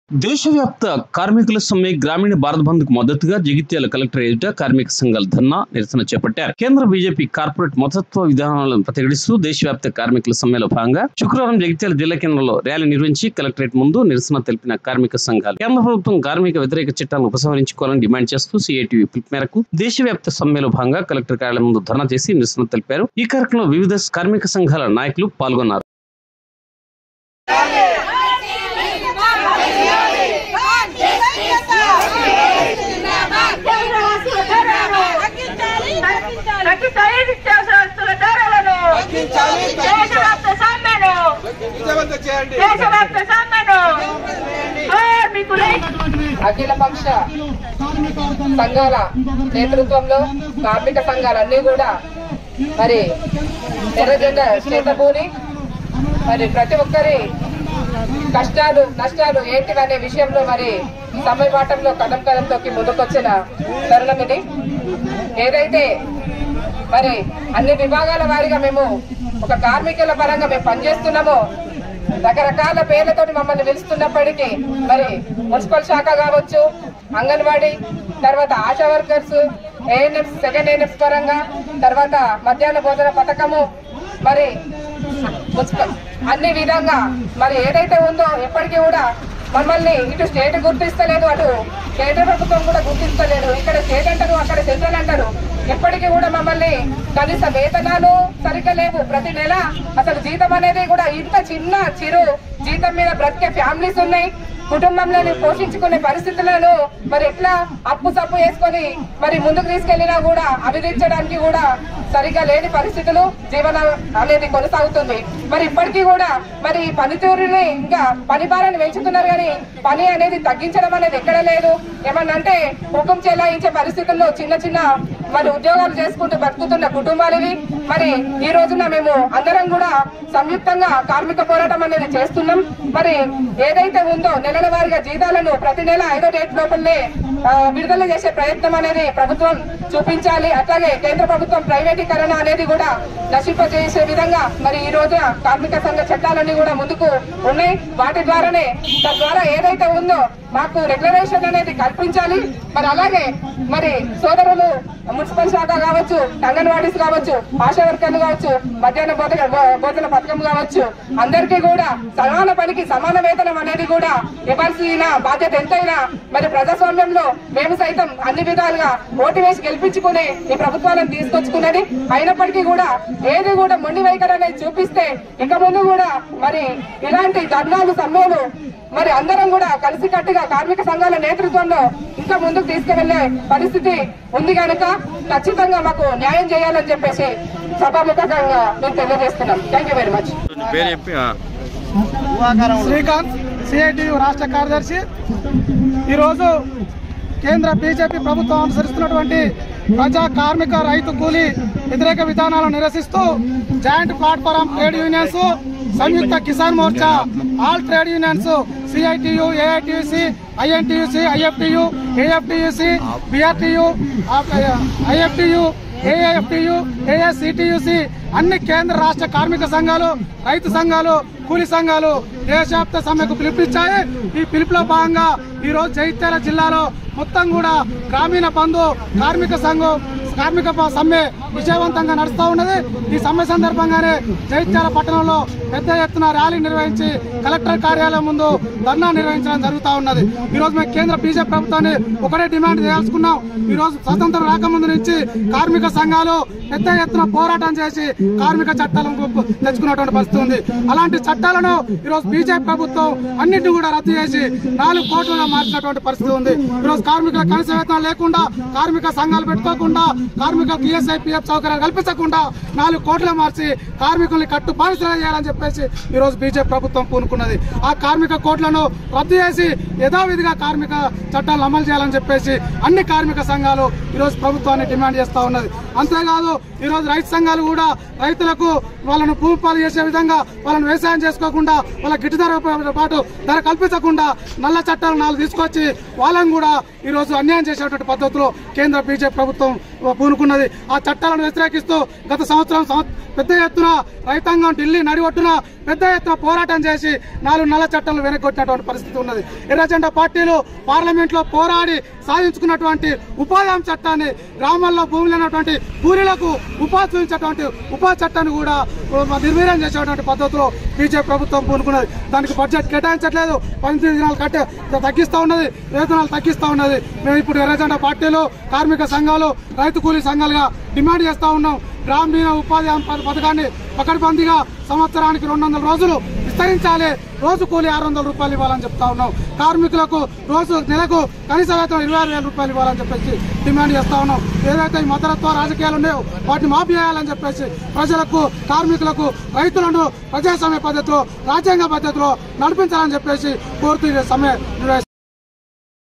16.2.24| DSC | Jagtial |దేశవ్యాప్త గ్రామీణ బంద్ కు మద్దతుగా కలెక్టరేట్ కార్మికుల ధర్నా
| Durga Siti Cable | News |దేశవ్యాప్త గ్రామీణ బంద్ కు మద్దతుగా కలెక్టరేట్ కార్మికుల ధర్నా